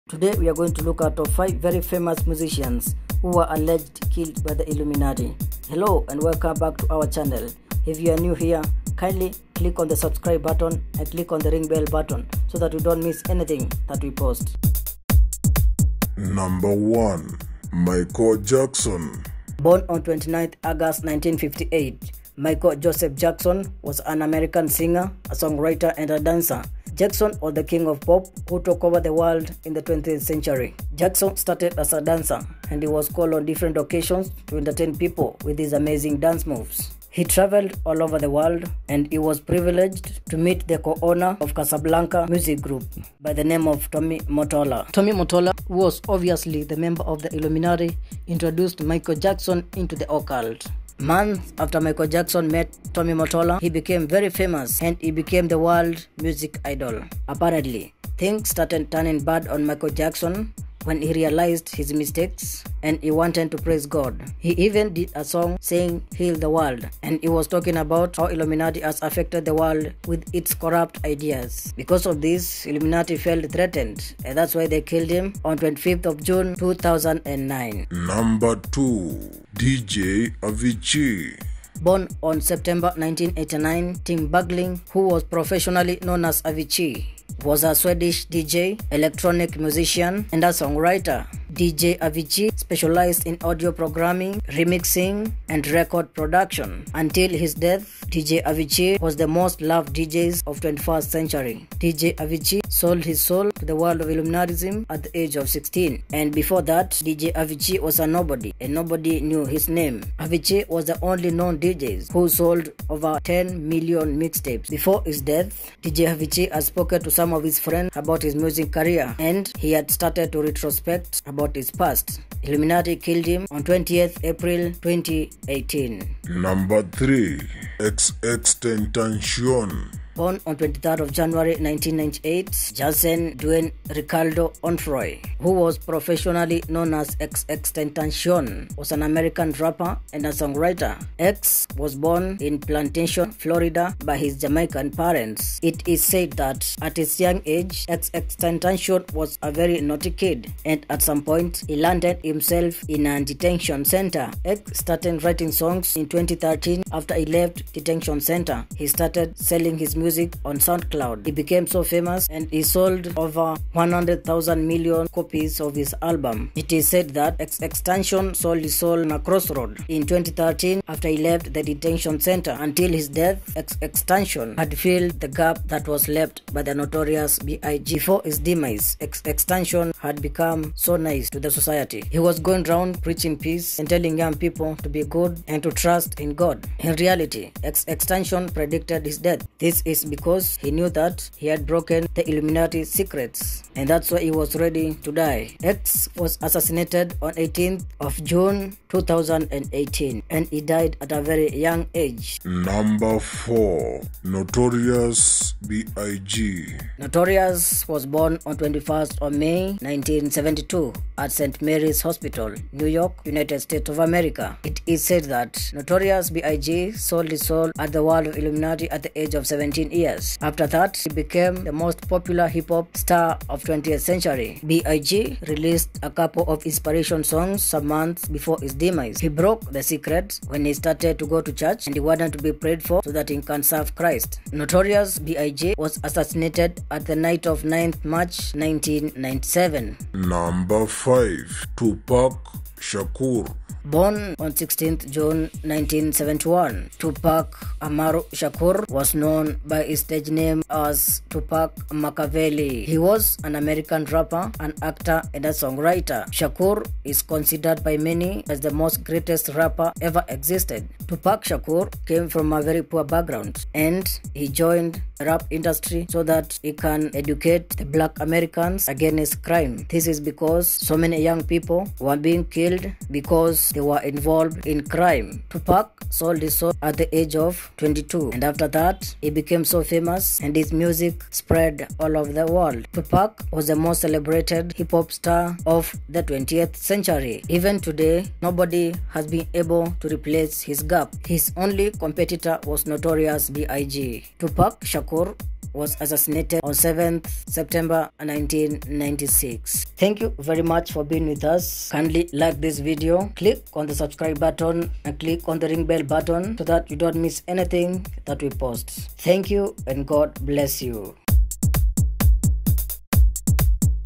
5 VERY FAMOUS MUSICIANS WHO WERE K!LLED BY ILLUMINATI
These are top fife very famous musicians, who were k!lled by Illuminati.  O the List we have Michael Jackson, Xxxtentacion , Avicii, Notorious B.I.G Etc.
Everyday People want to join Illuminati because of money power and wealth. Just trust in God instead of selling your soul to the Devil